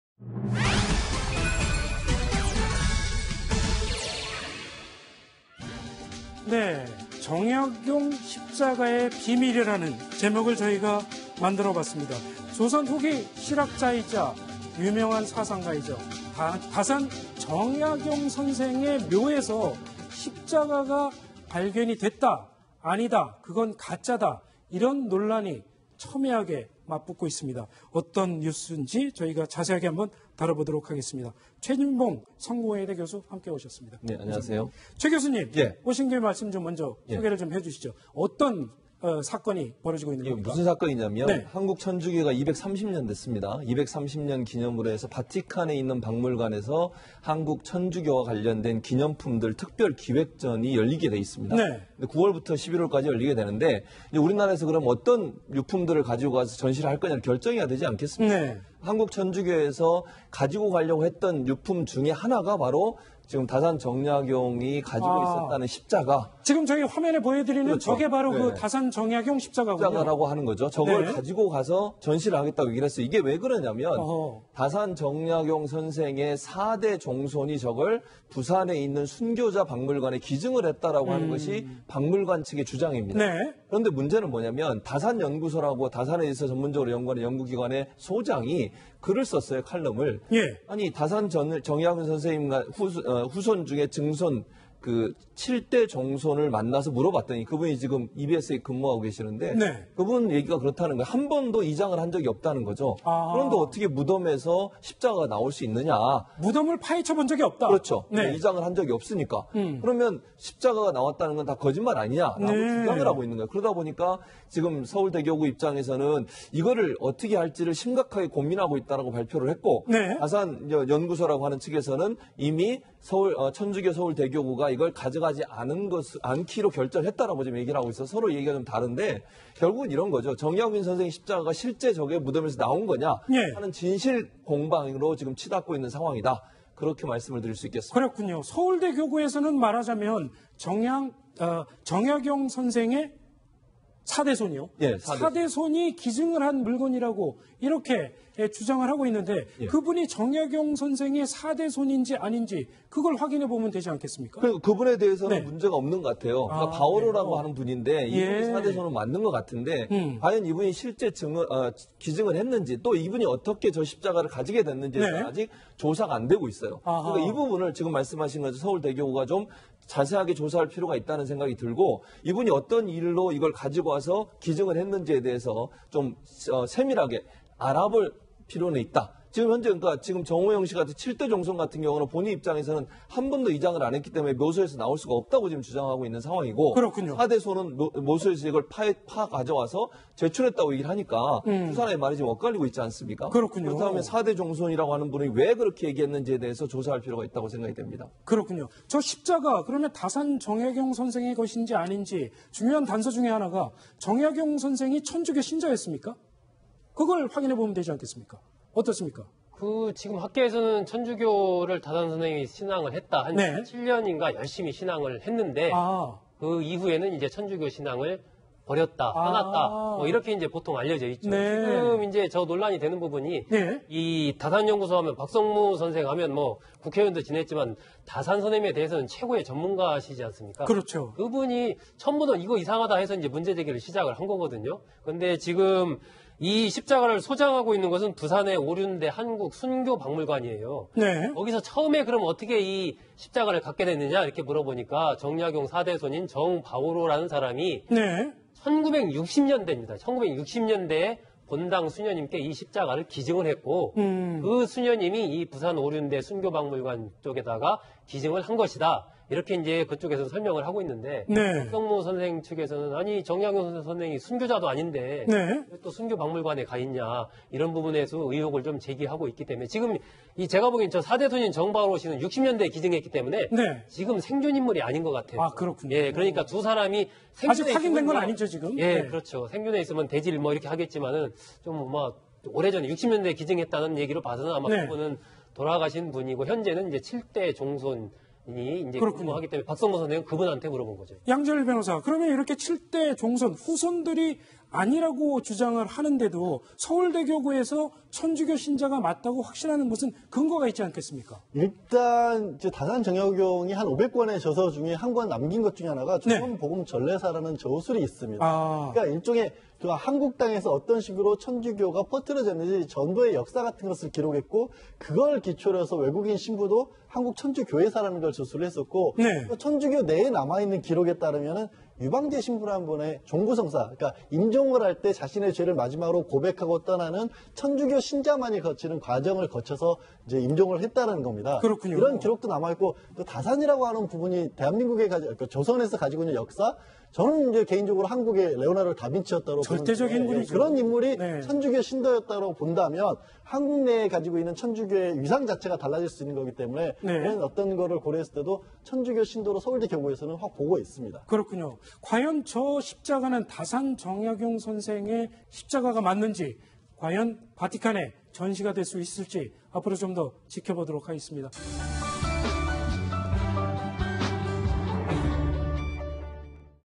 ‘정약용 십자가’의 비밀 / 다산 정약용의 십자가?···진위 논란
다산 정약용의 십자가?···진위 논란
다산 정약용 무덤서 나온 십자가···진실은?
무덤서 나왔다는 정약용 십자가 '진품' 논란
다산연구소·후손 "이장·파묘 없었는데 어떻게…"
정약용 십자가, 바티칸 못 간다···왜?
다산연구소 "정약용 종손, 십자가 확인 안 된 것"